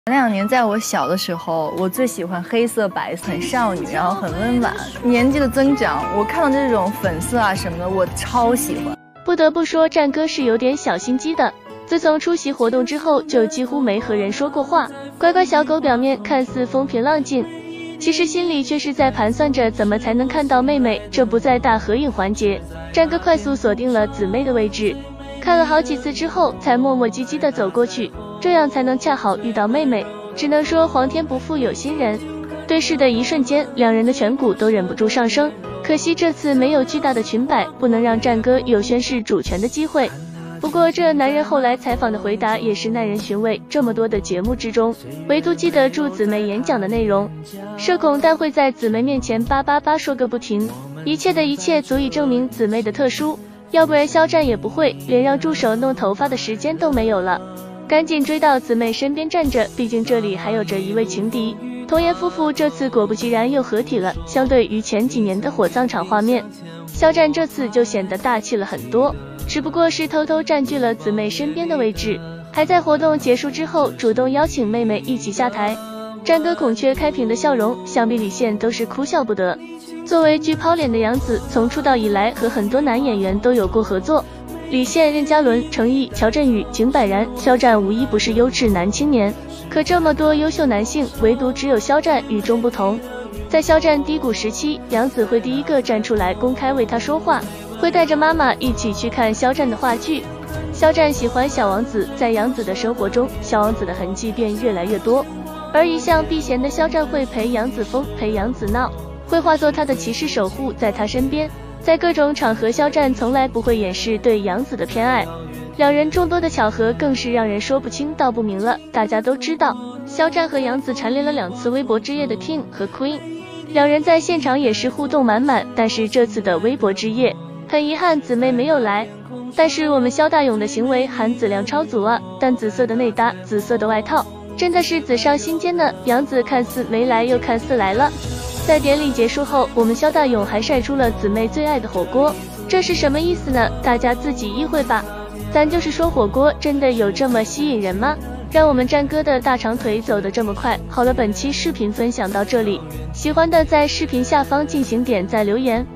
肖戰微博晚會有多勇？楊紫未到似到，現場含紫量超標，網友磕瘋啦，註意這孔雀開屏的微笑，和李現宣示主權#肖戰#楊紫#微博晚會#肖戰楊紫
肖战喜欢小王子，在杨紫的生活中，小王子的痕迹便越来越多，而一向避嫌的肖战会陪杨紫疯，陪杨紫闹，会化作他的骑士守护在她身边，在各种场合肖战从来不会掩饰对杨紫的偏爱，两人众多的巧合更是让人说不清道不明了，大家都知道，肖战和杨紫蝉联了两次微博之夜的king和queen，两人在现场也是互动满满，但是这次的微博之夜，很遗憾紫妹没有来，但是我们肖大勇的行为含紫量超足啊，淡紫色的内搭，紫色的外套，真的是紫上心间呢，杨紫看似没来又看似来了，在典礼结束后，我们肖大勇还晒出了紫妹最爱的火锅，这是什么意思呢，大家自己意会吧